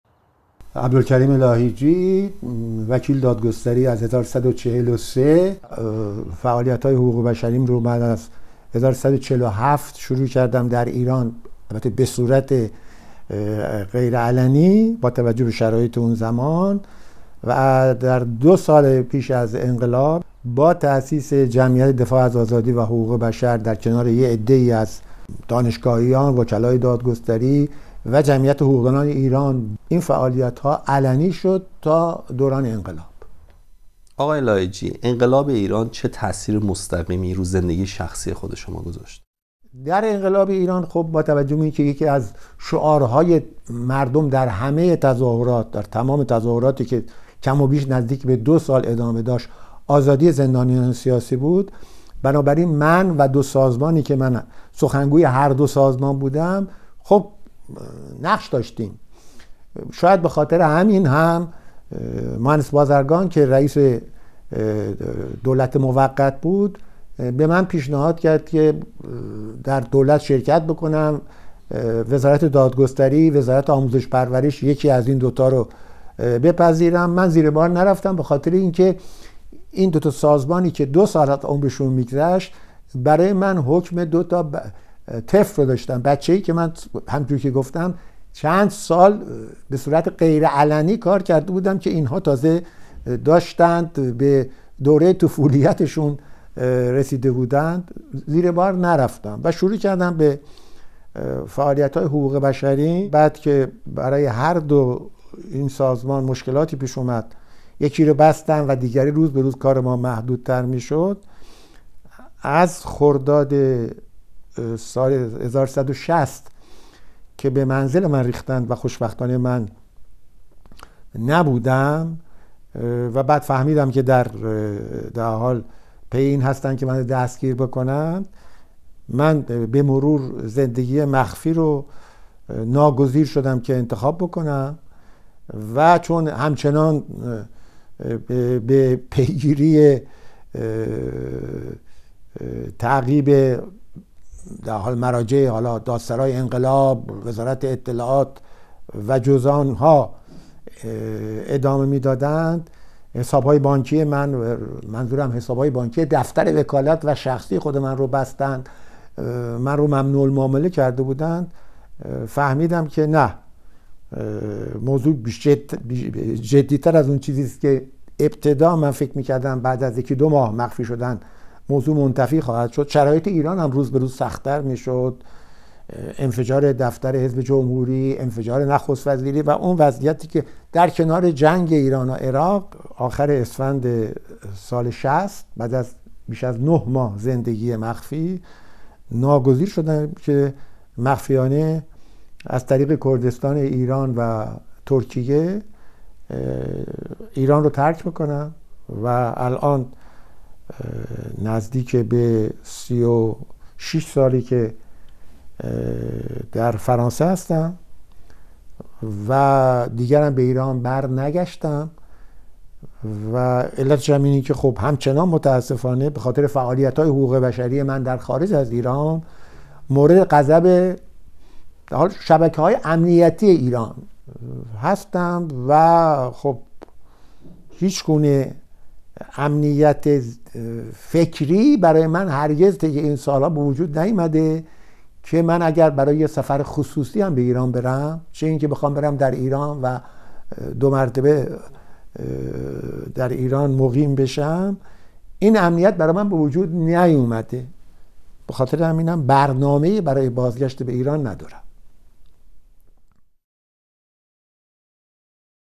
کریم لاهیجی: می‌توانستم وزیر شوم، اما حقوق بشر برایم مهم بود
کریم لاهیجی، حقوقدان و فعال حقوق بشر  می‌گوید که در اول انقلاب امکان وزیر شدن  را داشت، اما چه شد که مجبور به ترک ایران شد؟ گفت‌وگوی پوریا ماهرویان را با او ببینید.